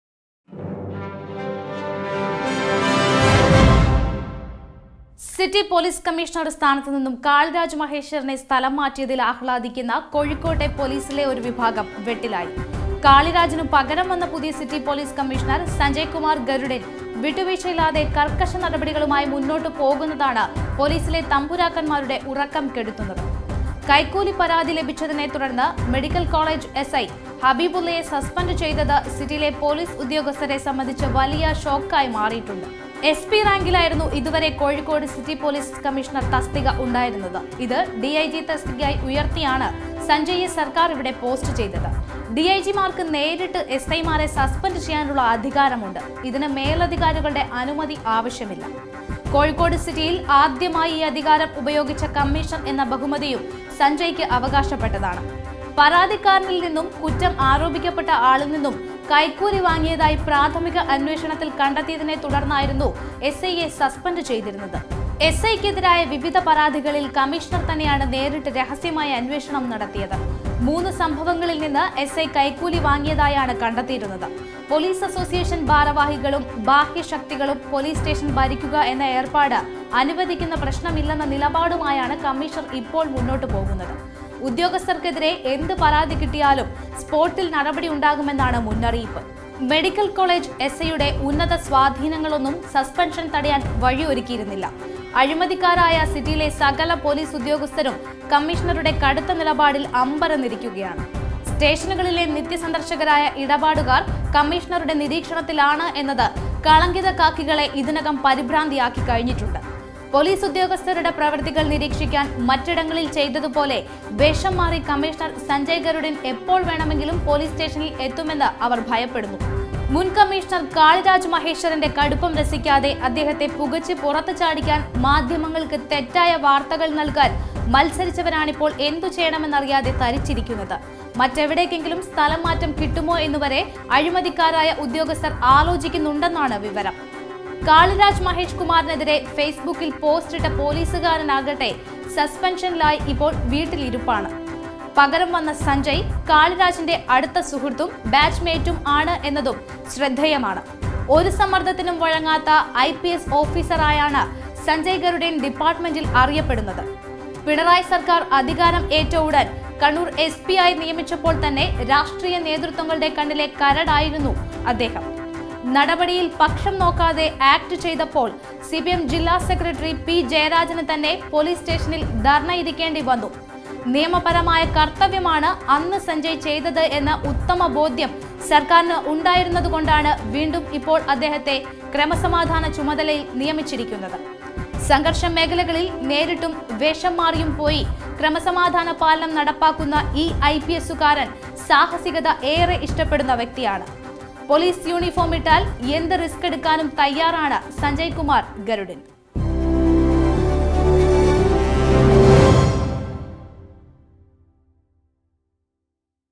പുലി' വേട്ട' തുടങ്ങി | Express Kerala
കാളിരാജിനു പകരം വന്ന പുതിയ സിറ്റി പൊലീസ് കമ്മീഷണര്‍ സഞ്ജയ് കുമാര്‍ ഗരുഡിന്‍ വിട്ടുവീഴ്ചയില്ലാതെ കര്‍ക്കശ നടപടികളുമായി മുന്നോട്ട് പോകുന്നതാണ് പൊലീസിലെ 'തമ്പുരാക്കന്‍മാരുടെ' ഉറക്കം കെടുത്തുന്നത്.